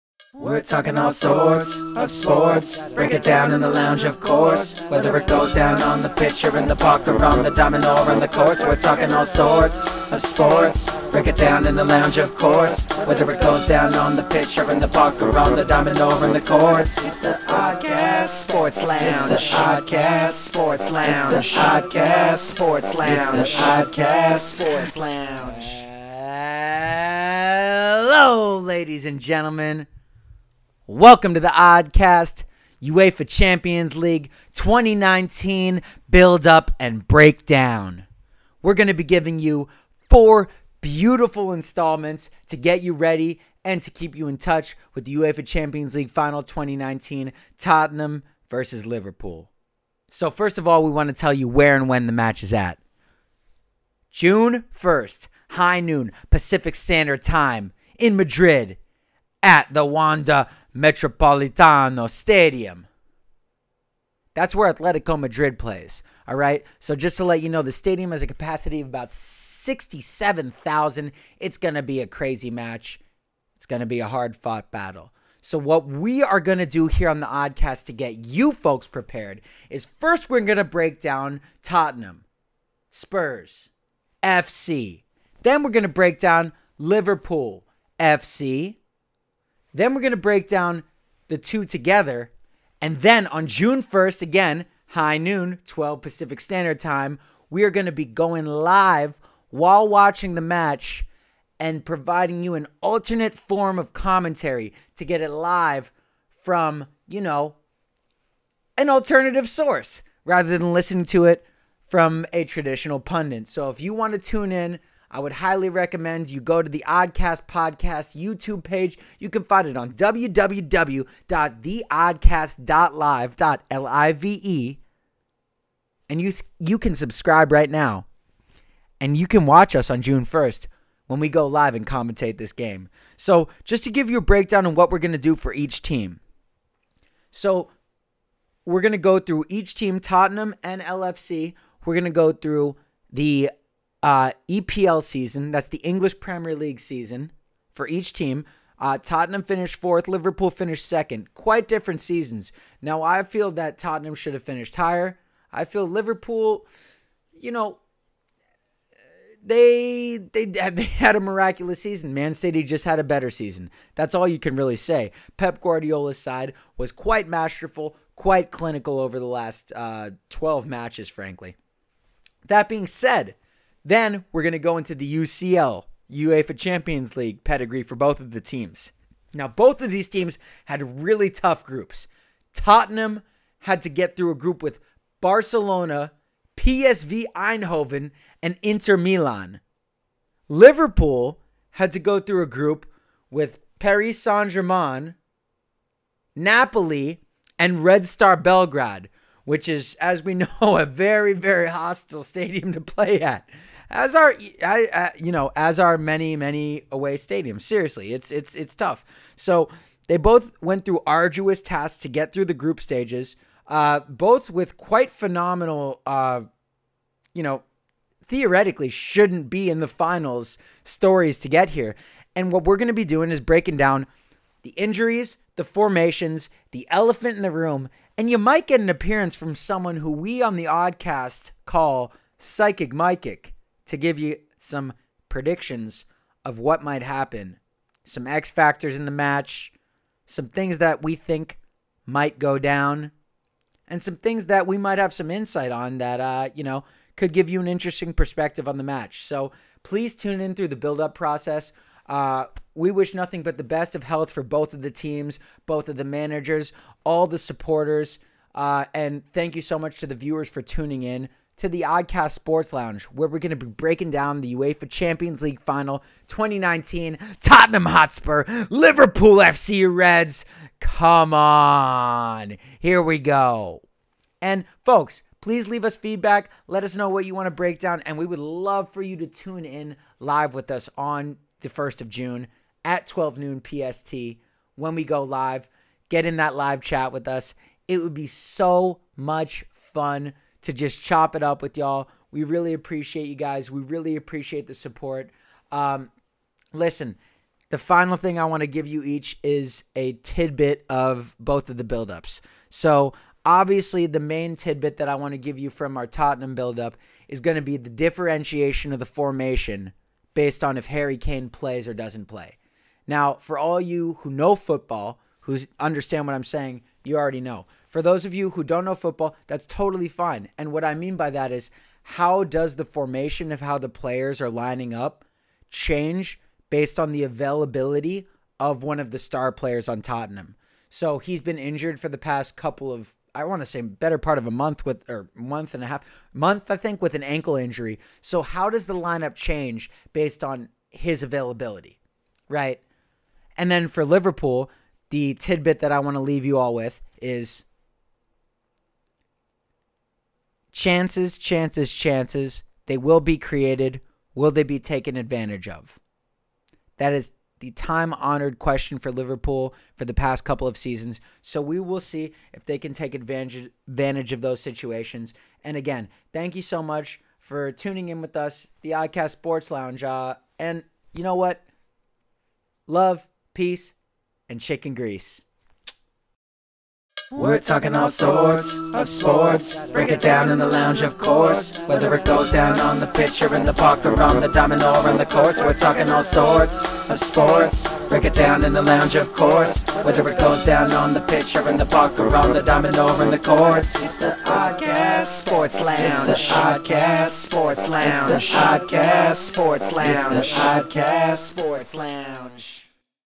2019 Champions League Final Buildup & Breakdown Liverpool FC v. Spurs: INTRO
This is the intro to our 4 part coverage of the 2019 Champions League Final between Liverpool FC and Tottenham FC.The First Segment will cover the Spurs.The Second Segment will cover Liverpool.The Penultimate Segment will cover both sides and compare the two.The final segment will be a live streaming commentary conducted while the match is being played!